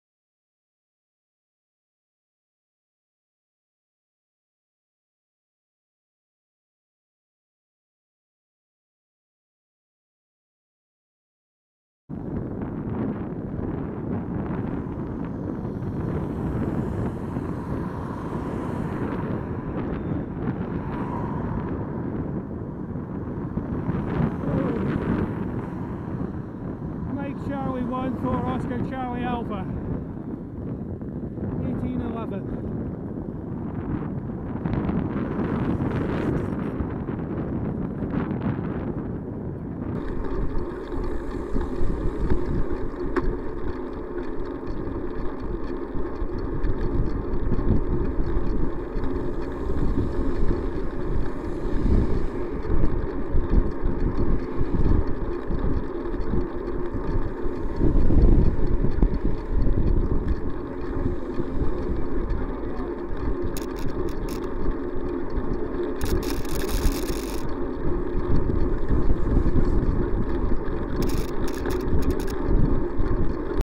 MC14OCA overtake into oncoming traffic
Driver doesn't appear to be in a hurry yet on a straight road with good visibility and a clearly visible oncoming car they cross into the right hand lane and drive head on at the oncoming car.